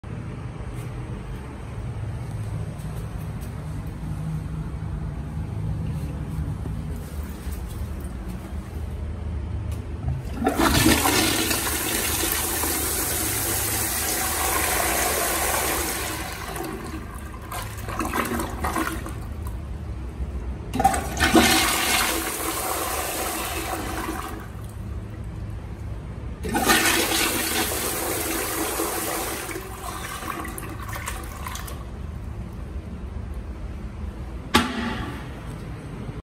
Cotto C1322BI Munich back spud inwall flushometer toilet paper test (Part 2)
This is a paper test on a Cotto C1322BI Munich back spud inwall flushometer toilet with the Cotto CT456NL flushometer. As it has a powerful flush it took down this huge amount of paper very well without clogging it which surprised me. This toilet passed the test very well.